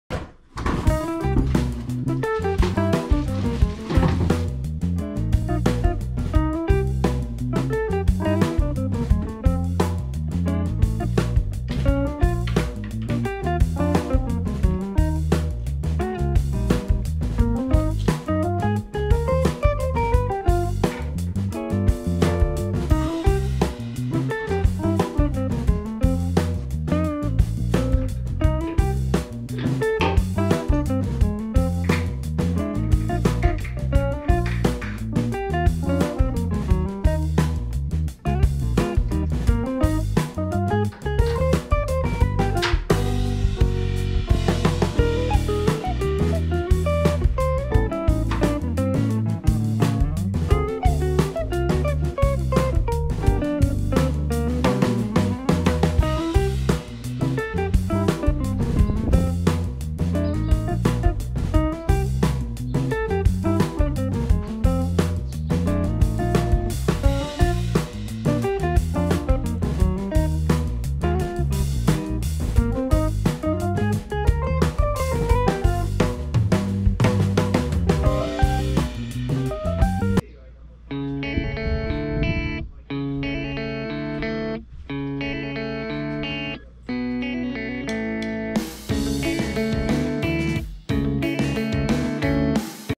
【Smartmi スマート扇風機S2】コードレスでスマホ操作可能な超絶シンプルデザイン扇風機を紹介
扇風機を購入しました。他にないシンプルなデザイン、コードレス、スマホ操作が気に入ったので、紹介します。